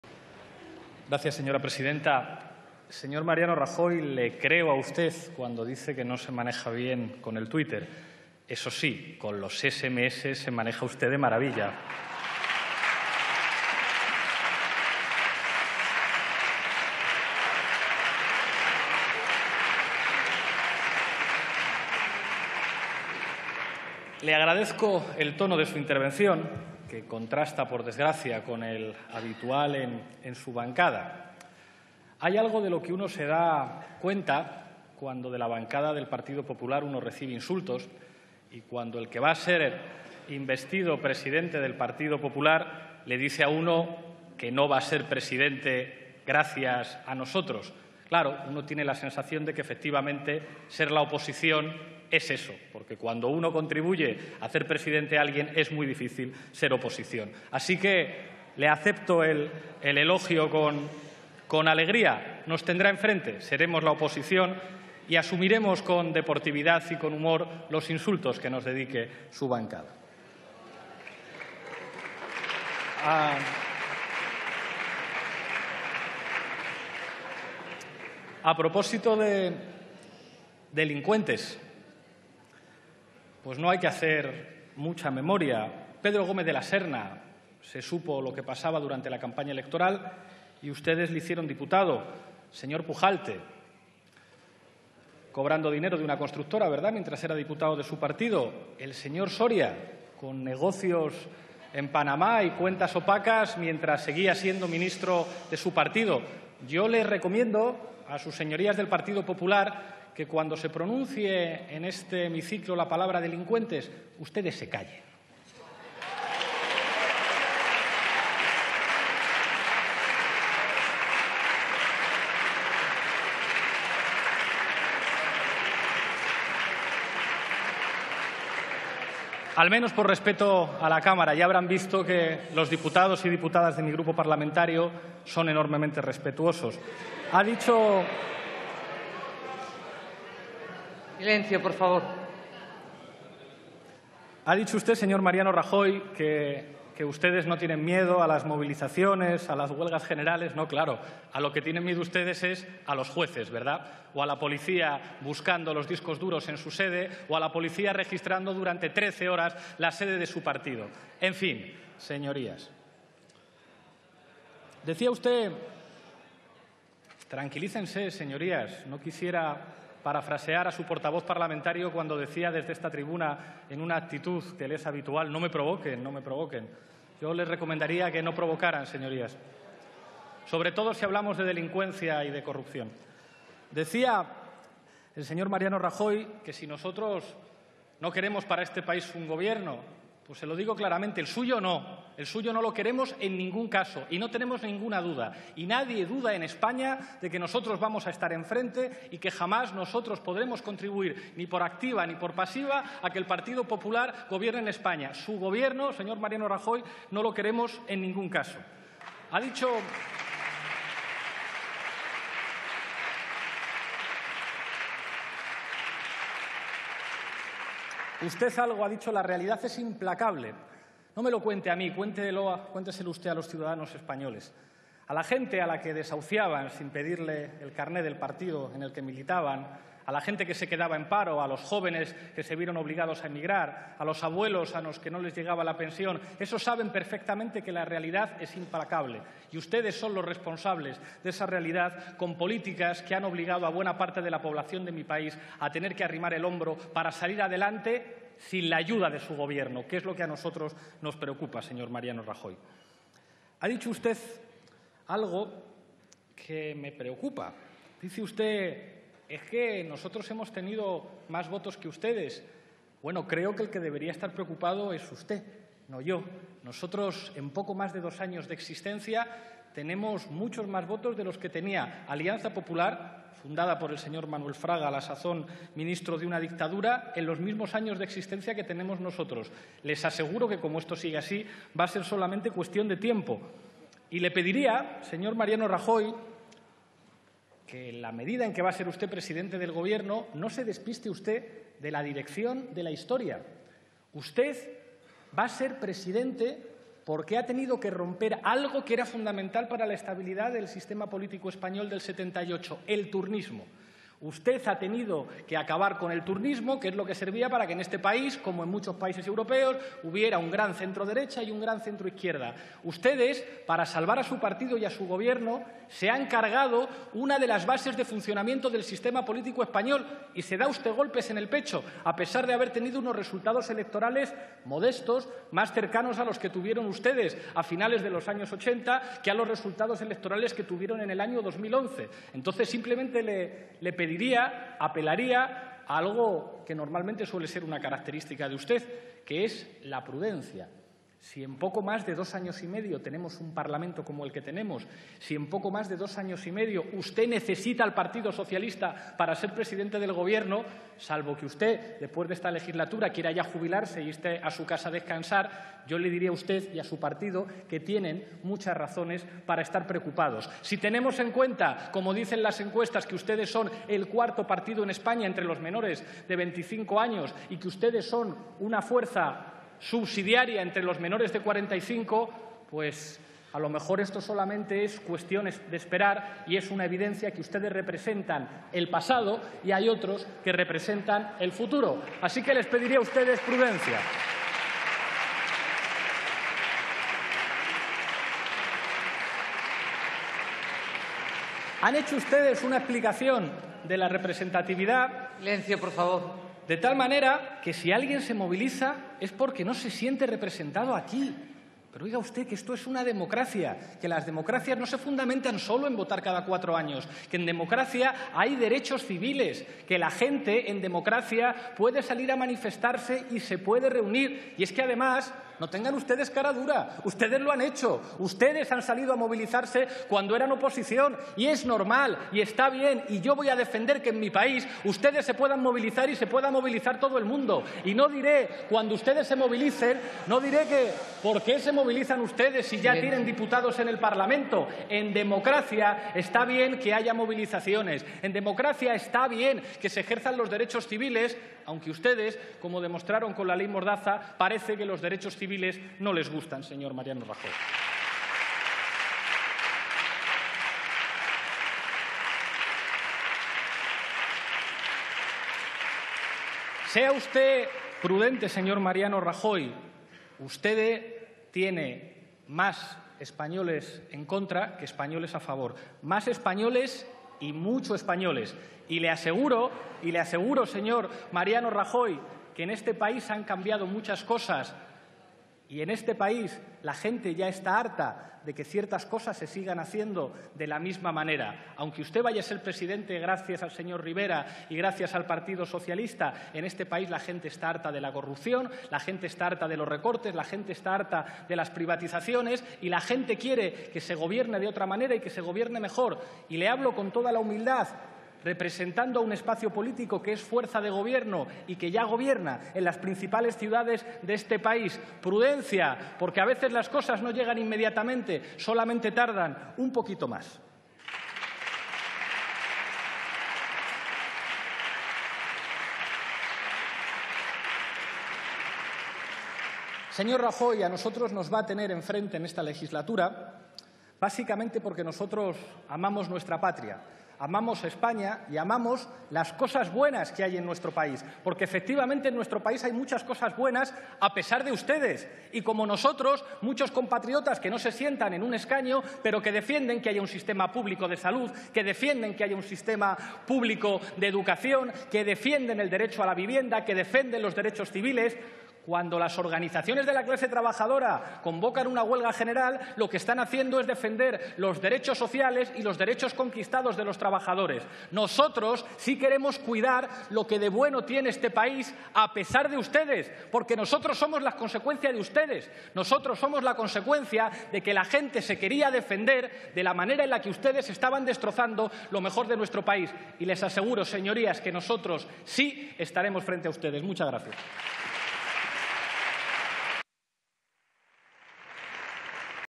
Pablo Iglesias: "El PP nos tendrá enfrente porque amamos a nuestra patria y queremos defenderla"
Segunda intervención de Pablo Iglesias en la sesión de investidura el 27 de Octubre de 2016.